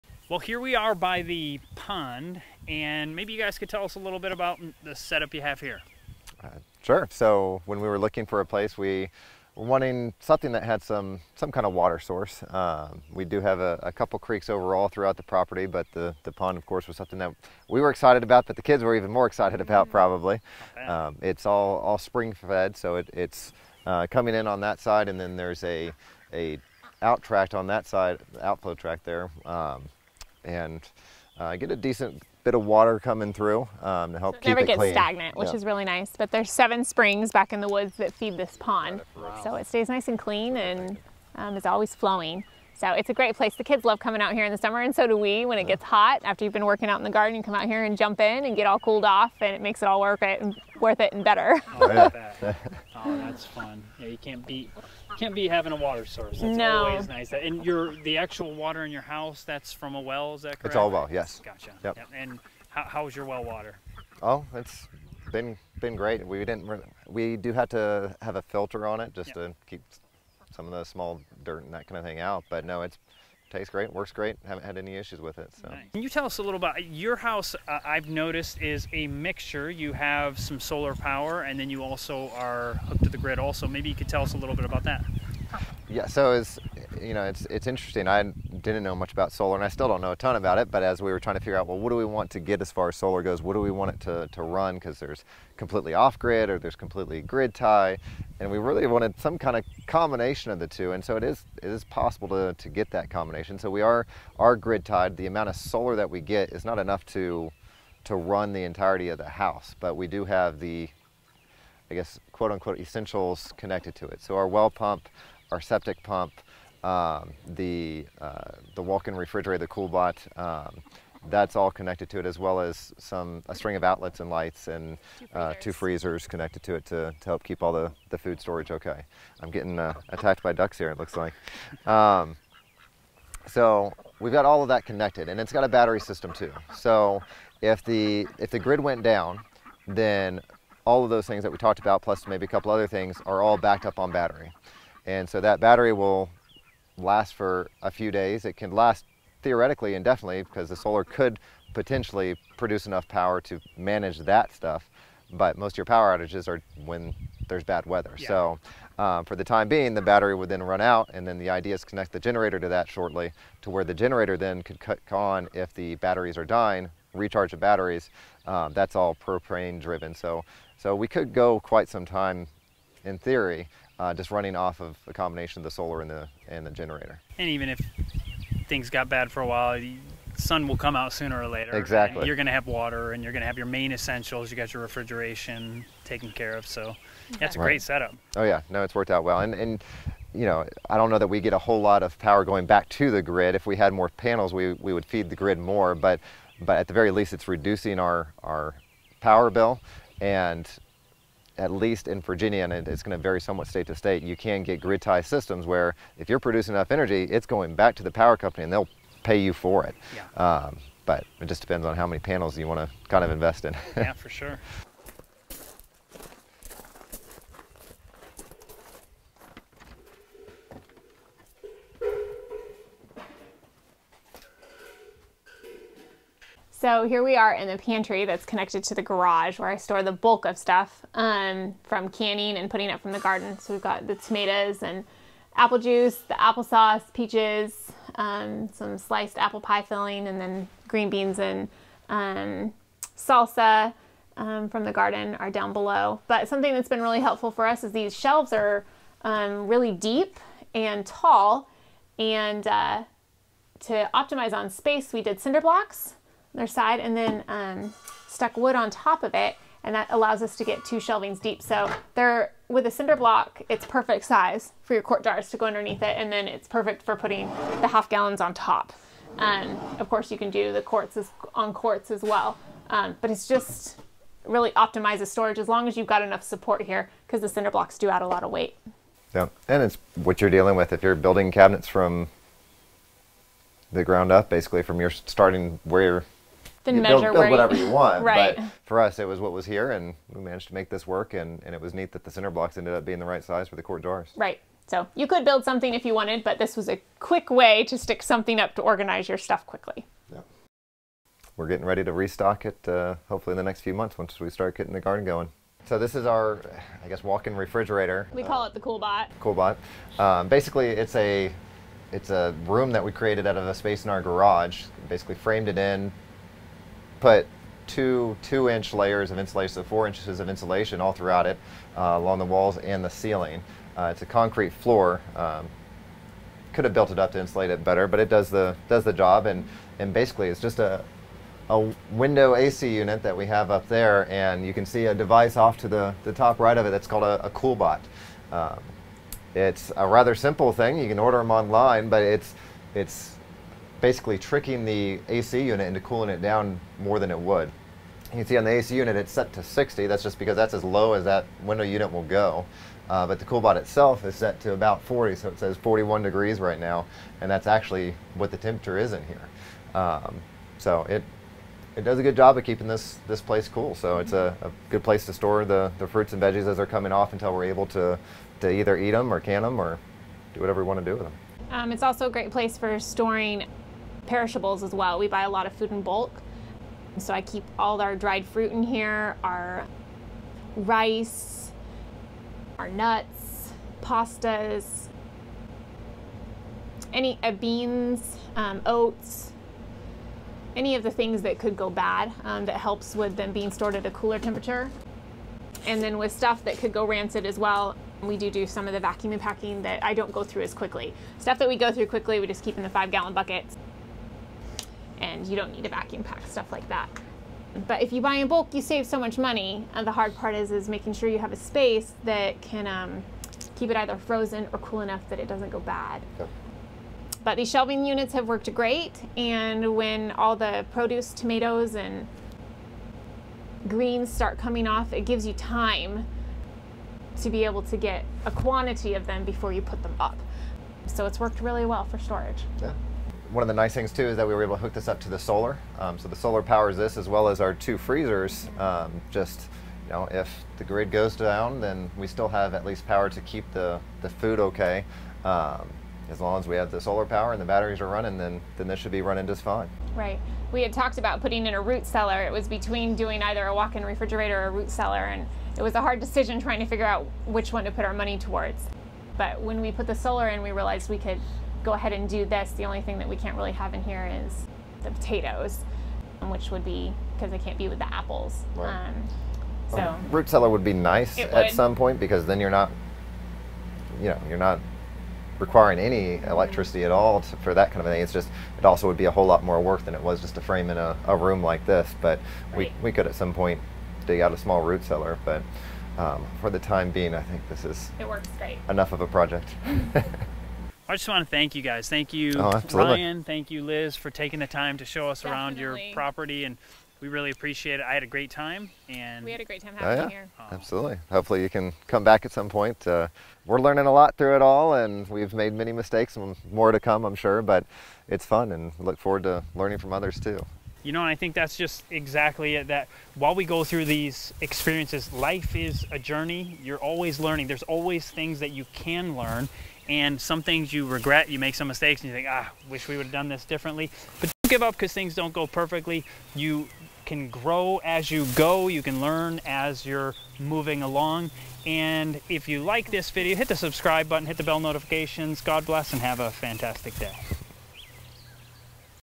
Tips on Solar power (Grid Tied) Cold Storage and Canning Storage
Have you ever wondered about solar power that is grid tied? Here is a personal experience with it and also some simple tips on storing your canned veggies and fruit. Additionally we have info on how to turn a window air conditioner into a room refrigerator.